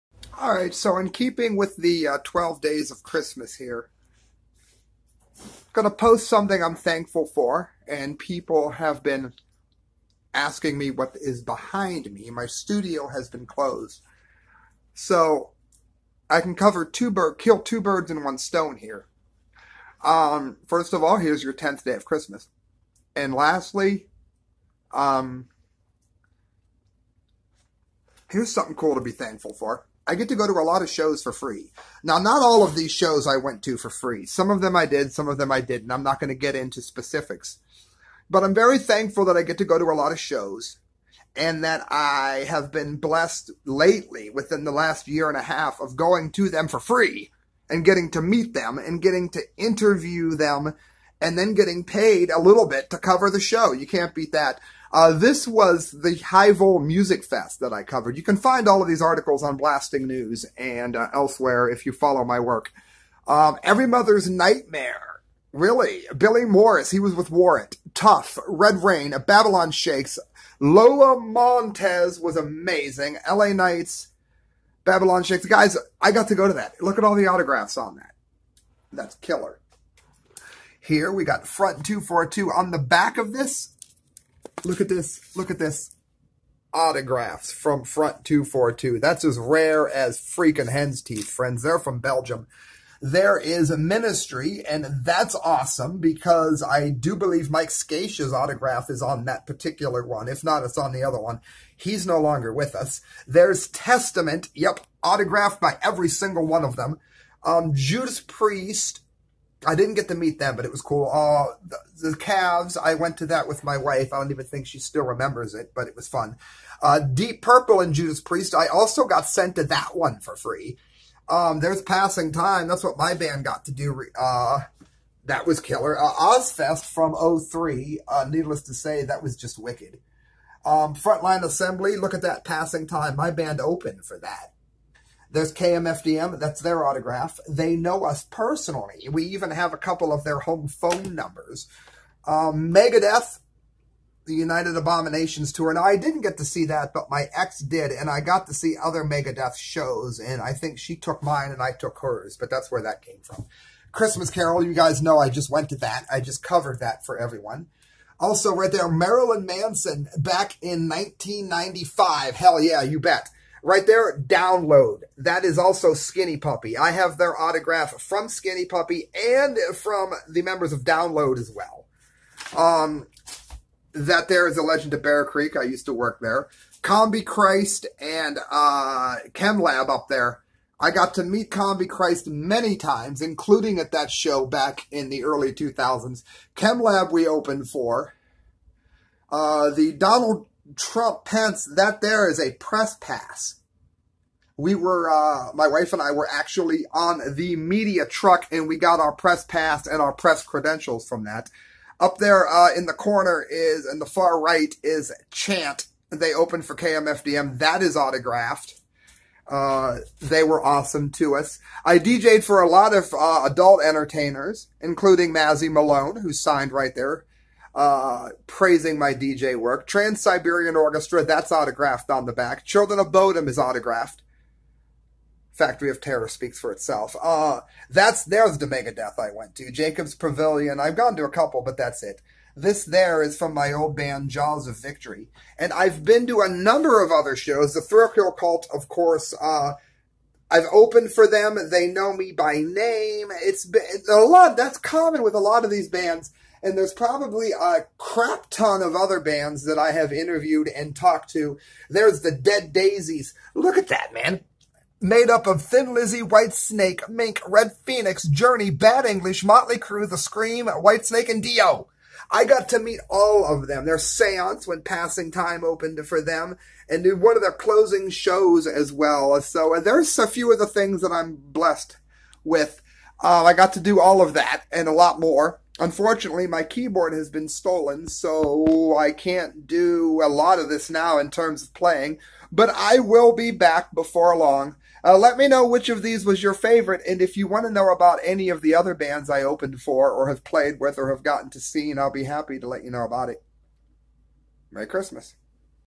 Day 10 - The "Wall Of Thankful" - THE CORRECT VIEWS 12/24/2018
'Tis Sam "I Be" Di Gangi, a freelance writer for WitsNews, Blasting News, and The Media Speaks with a look at the "WALL OF THANKFUL" that so many of you have asked about while my studio is down.
~Sam